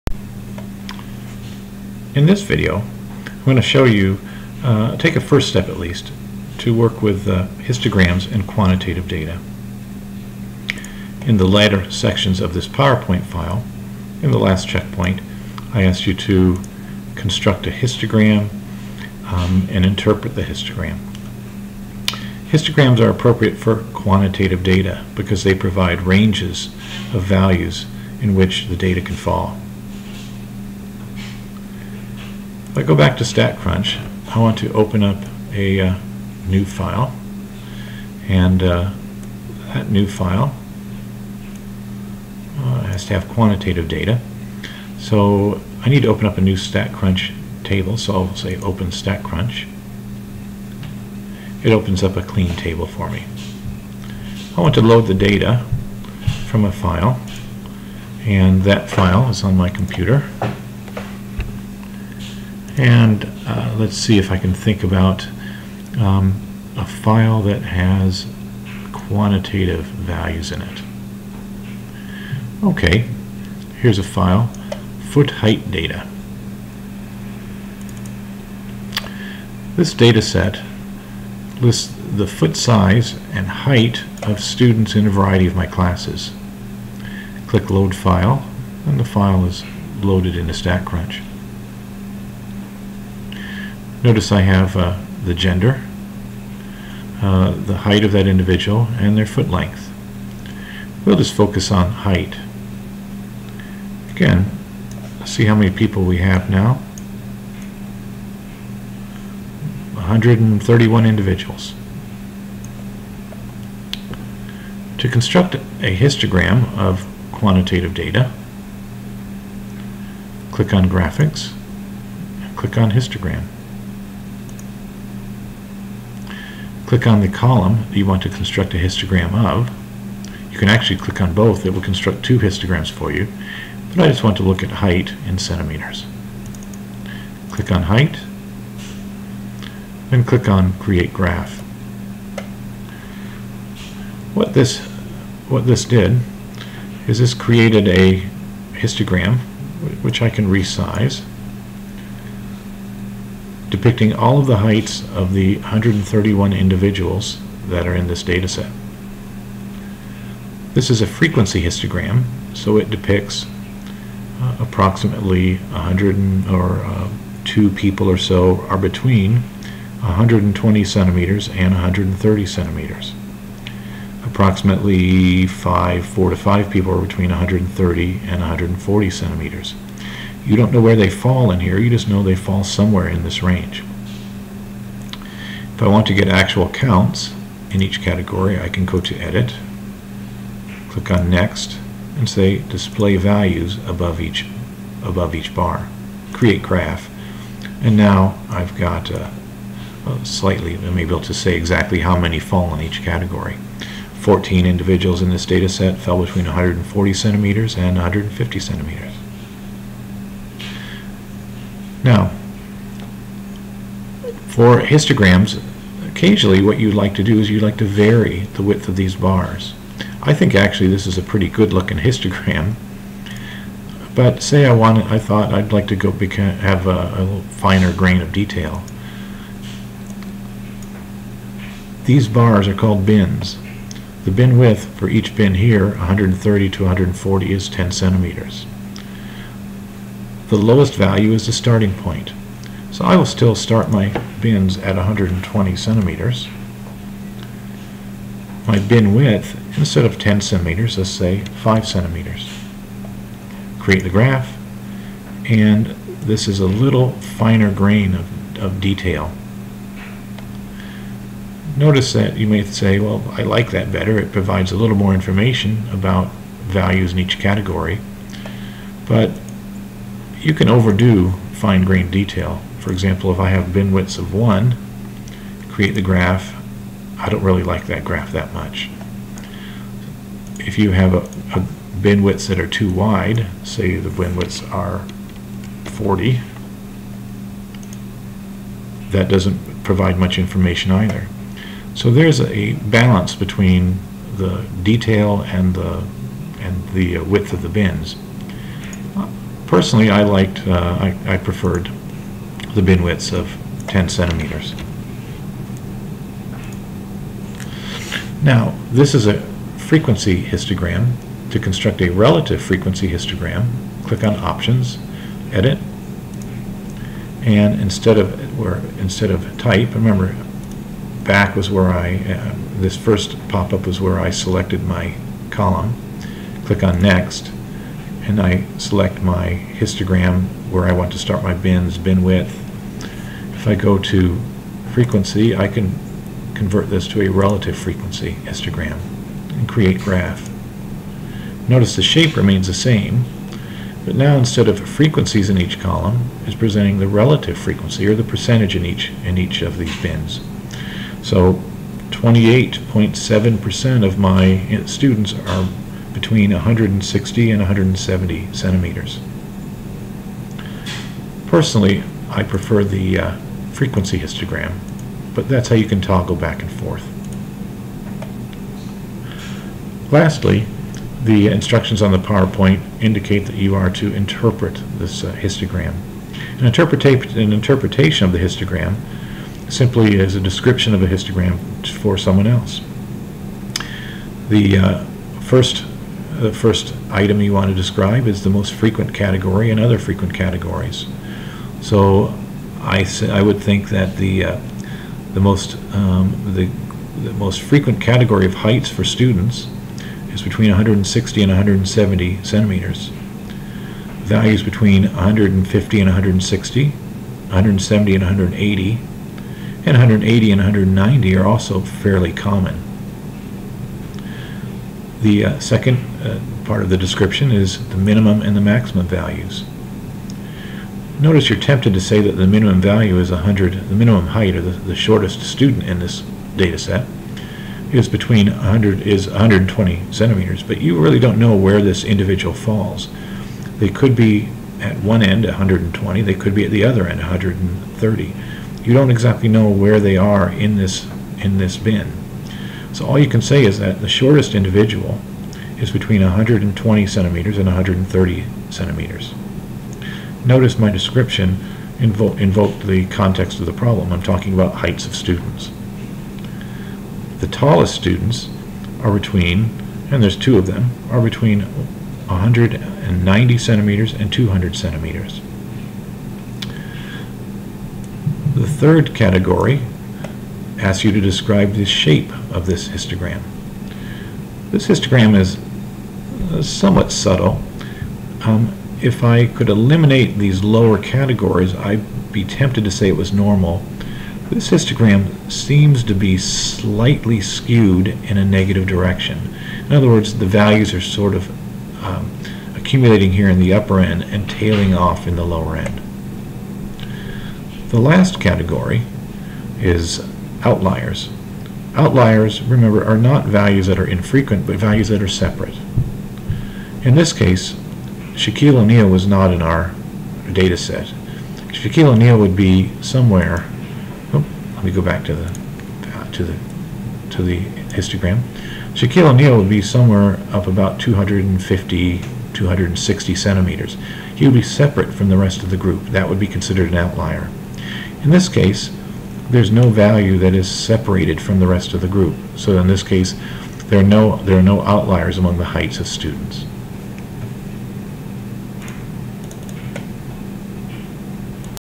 representing quantitative data
Using statcrunch to construct a histogram of quantitative data, interpreting the histogram